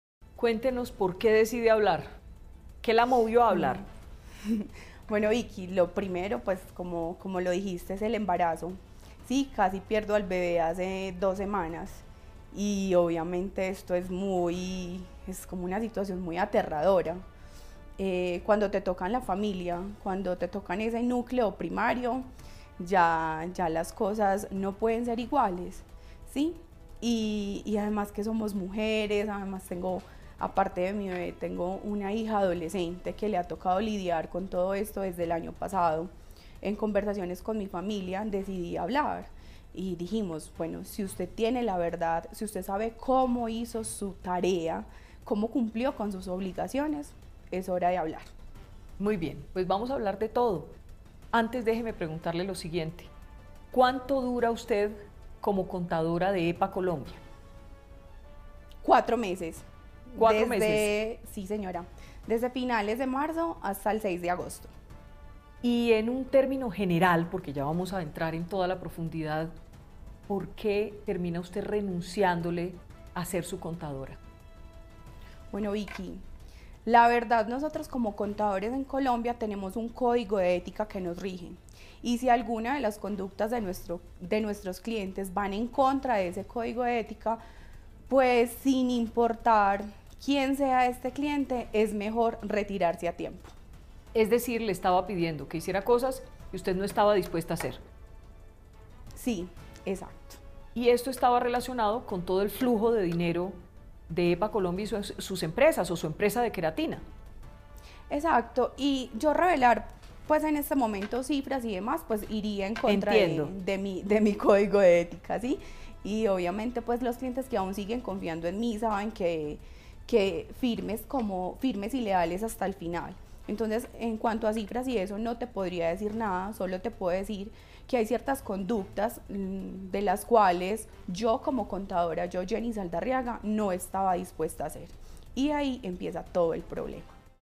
Contadora de Epa Colombia revela por qué renunció a su trabajo | Semana Noticias
"Tenemos un código de ética": Yenny Saldarriaga, en EXCLUSIVA para SEMANA, cuenta por qué renunció a seguir trabajando como contadora de Daneidy Barrera Rojas, mejor conocida como Epa Colombia.